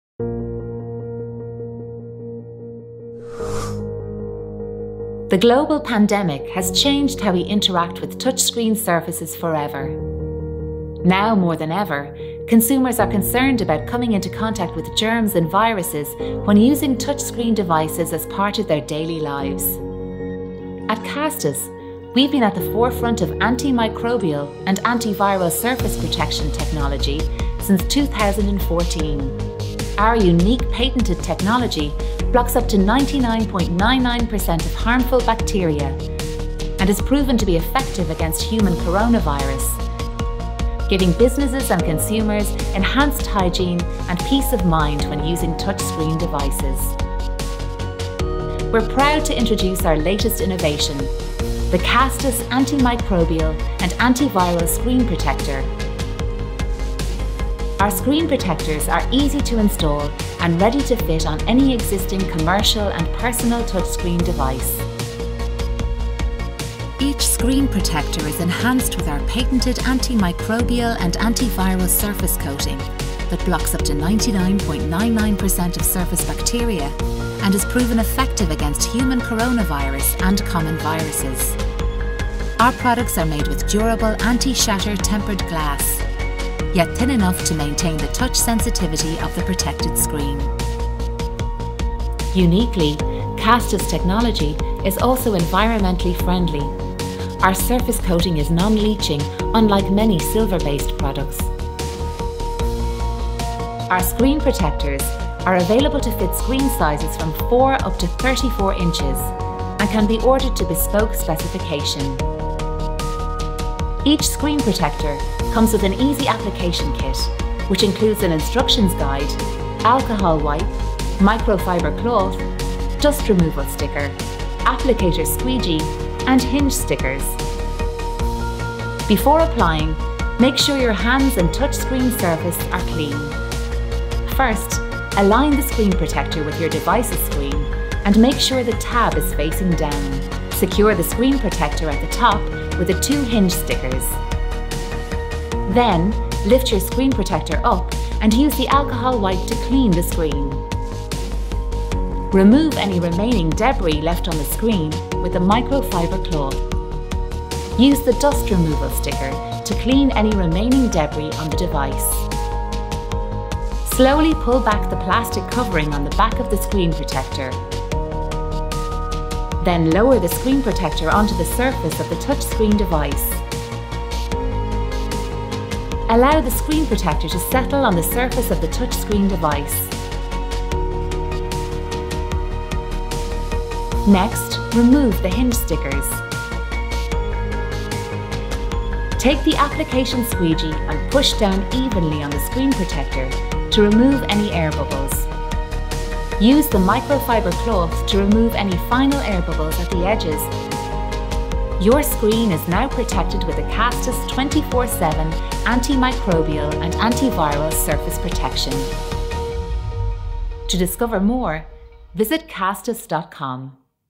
Kastus® Antimicrobial and Antiviral Screen Protectors Application Video
We produced this application video for our client Kastus to showcase their new revolutionary product and explain the application process of the product onto most screens.

From pre-production to production and post production we completed the job in house including Voice Over artist recording, recording in our studio in Dublin, video animations and video editing.